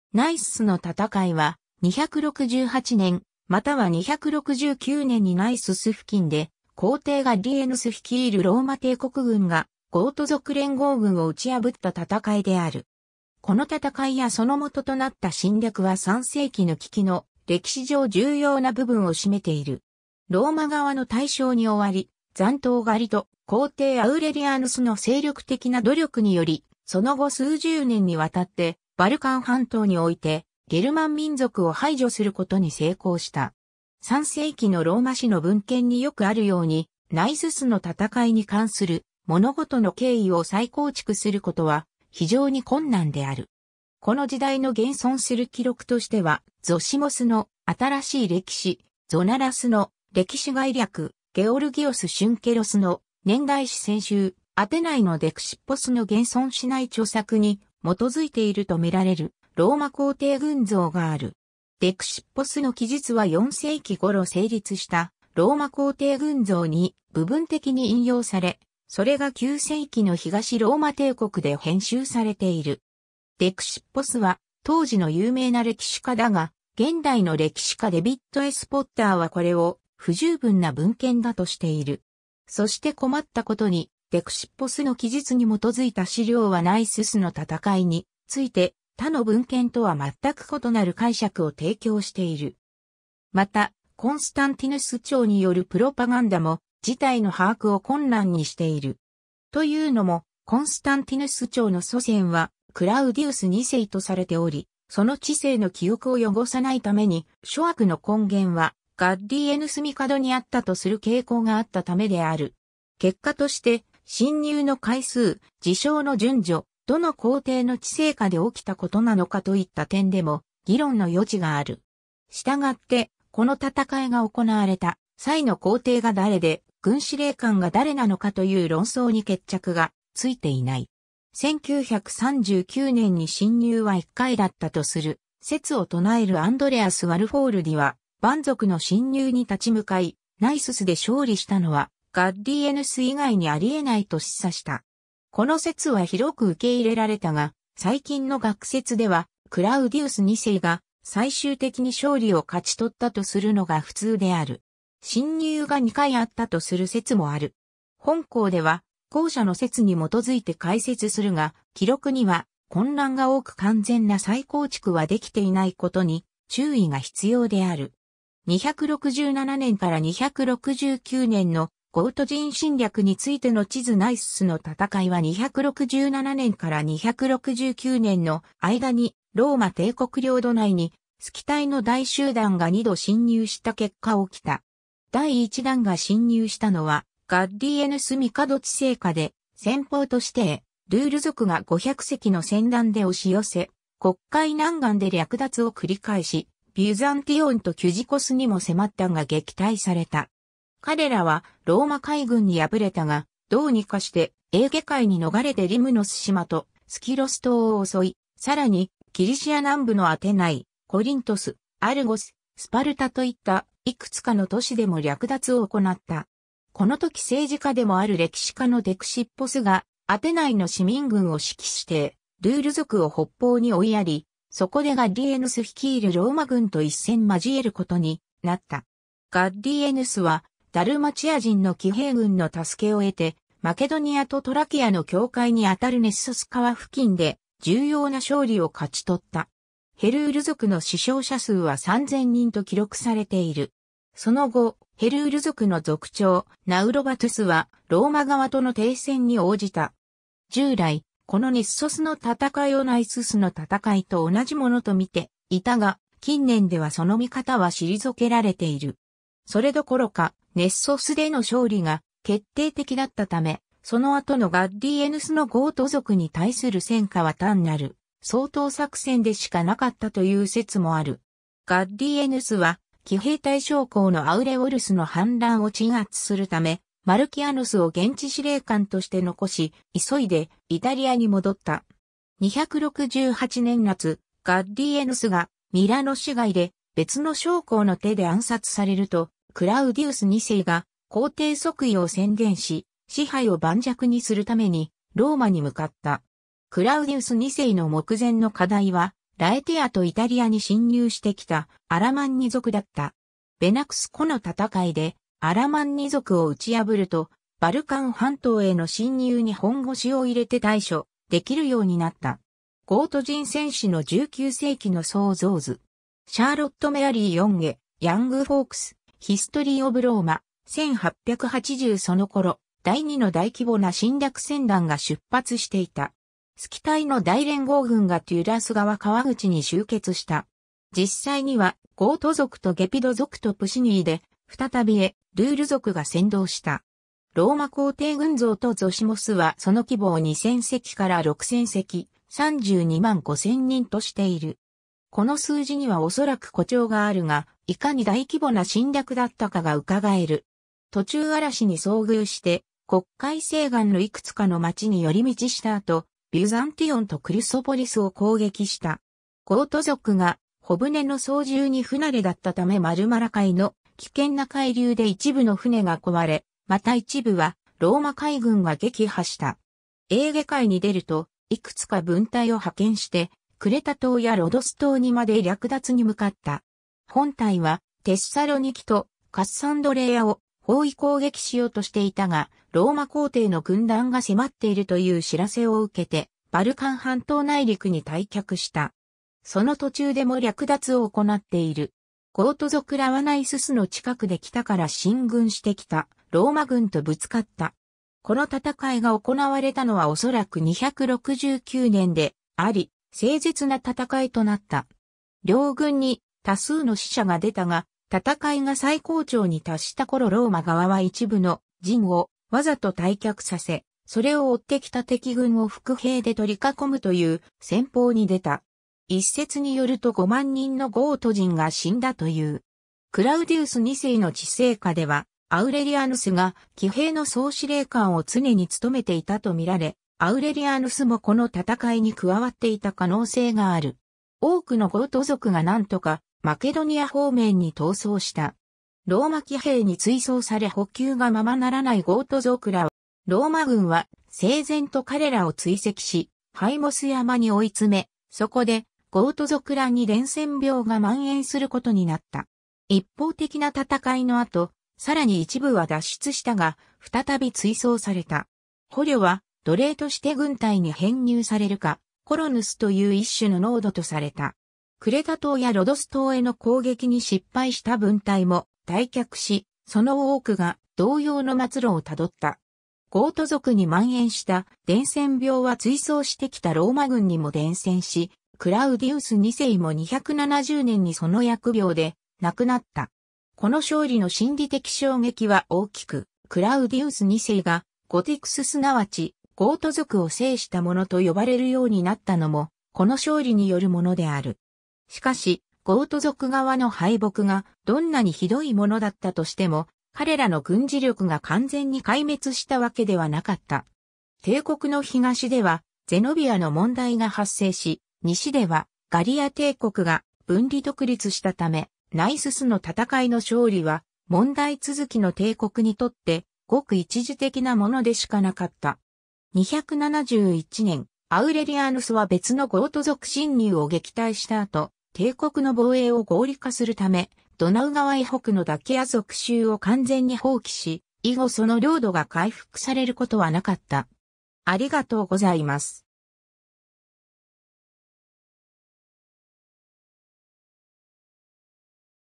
ナイススの戦い
ナイススの戦い（ナイススのたたかい、英：Battle of Naissus）は、268年または269年にナイスス（現在のセルビアのニシュ）付近で皇帝ガッリエヌス（またはクラウディウス2世）率いるローマ帝国軍がゴート族連合軍を打ち破った戦いである。
この戦いやその元となった侵略は3世紀の危機の歴史上重要な部分を占めている。
 ローマ側の大勝に終わり、残党狩りと皇帝アウレリアヌスの精力的な努力により、その後数十年に渡ってバルカン半島においてゲルマン民族を排除することに成功した。
 3世紀のローマ史の文献によくあるように、ナイススの戦いに関する物事の経緯を再構築することは非常に困難である。
この時代の現存する記録としては、ゾシモスの『新しい歴史』、ゾナラスの『歴史概略』、ゲオルギオス・シュンケロスの『年代誌選集』、アテナイのデクシッポスの現存しない著作に基づいていると見られる『ローマ皇帝群像』がある。
デクシッポスの記述は4世紀ごろ成立した『ローマ皇帝群像』に部分的に引用され、それが9世紀の東ローマ帝国で編集されている。
デクシッポスは当時の有名な歴史家だが、現代の歴史家 David S. Potter はこれを「不十分な」文献だとしている。
そして困ったことに、デクシッポスの記述に基づいた資料はナイススの戦いについて他の文献とは全くことなる解釈を提供している。
また、コンスタンティヌス朝によるプロパガンダも事態の把握を困難にしている。
というのもコンスタンティヌス朝の祖先はクラウディウス2世とされており、その治世の記憶を汚さないために、諸悪の根源はガッリエヌス帝にあったとする傾向があったためである。
 結果として、侵入の回数、事象の順序、どの皇帝の治世下で起きたことなのかといった点でも議論の余地がある。
したがって、この戦いが行われた際の皇帝が誰で、軍司令官が誰なのかという論争に決着がついていない。
1939年に侵入は1回だったとする説を唱える Andreas Alföldi は、蛮族の侵入に立ち向かいナイススで勝利したのはガッリエヌス以外にありえないと示唆した。
この説は広く受け入れられたが、最近の学説ではクラウディウス2世が最終的に勝利を勝ち取ったとするのが普通である。
侵入が2回あったとする説もある。
本項では後者の説に基づいて解説するが、記録には混乱が多く完全な再構築はできていないことに注意が必要である。
 267年から269年のゴート人侵略についての地図 ナイススの戦いは267年から269年の間にローマ帝国領土内にスキタイ（ここで参照している文献は時代錯誤的にこう呼んでいる）の大集団が2度侵入した結果起きた。
第1団が侵入したのはガッリエヌス帝治世下（267年）で、先鋒としてヘルール族が500隻の船団で押し寄せ、黒海南岸で略奪を繰り返し、ビュザンティオンとキュジコスにも迫ったが撃退された。
彼らはローマ海軍に敗れたが、どうにかしてエーゲ海に逃れてリムノス島とスキロス島を襲い、さらにギリシア南部（アカエア）のアテナイ、コリントス、アルゴス、スパルタといったいくつかの都市でも略奪を行った。
このとき政治家でもある歴史家のデクシッポスがアテナイの市民軍を指揮してヘルール族を北方に追いやり、そこでガッリエヌス率いるローマ軍と一戦交えることになった。
ガッリエヌスはダルマチア人の騎兵軍の助けを得て、マケドニアとトラキアの境界にあたるネッソス川（メスタ川）付近で重要な勝利を勝ち取った。
ヘルール族の死傷者数は3千人と記録されている。
その後、ヘルール族の族長ナウロバトゥスはローマ側との停戦に応じた。
 従来、このネッソスの戦いをナイススの戦いと同じものと見ていたが、近年ではその見かたは退けられている（実際、メスタ川のギリシア名はネストスであってネッソスではない）。
それどころか、ネッソスでの勝利が決定的だったため、その後のガッリエヌスのゴート族に対する戦果は（ナイススの戦いも含めて）単なる掃討作戦でしかなかったという説もある。
ガッリエヌスは騎兵隊将校のアウレオルスの反乱を鎮圧するため、マルキアヌスを現地司令官として残し、急いでイタリアに戻った。
268年夏、ガッリエヌスがミラノ市外で別の将校の手で暗殺されると、クラウディウス2世が皇帝即位を宣言し、支配を磐石にするためにローマに向かった。
クラウディウス2世の目前の課題は、ラエティアとイタリアに侵入してきたアラマンニ族だった。
ベナクス湖の戦い（現在のガル...